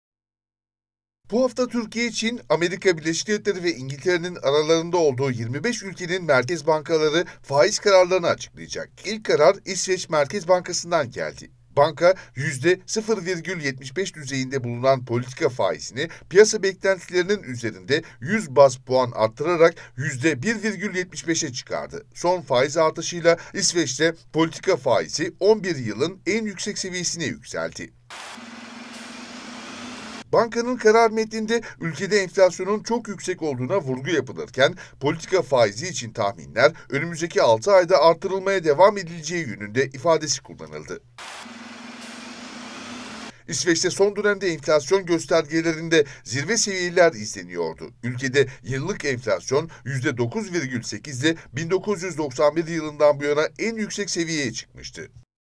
İsveç’ten tarihi faiz artırımı
İsveç Merkez Bankası piyasa beklentilerinin üzerinde 100 baz puan faiz artışına gitti. Bu, 1993'ten bu yana en yüksek faiz artırımı olarak kayda geçti. Ülkede enflasyon yüzde 9 ile 1991'den bu yana görülen en yüksek seviyede seyrediyor.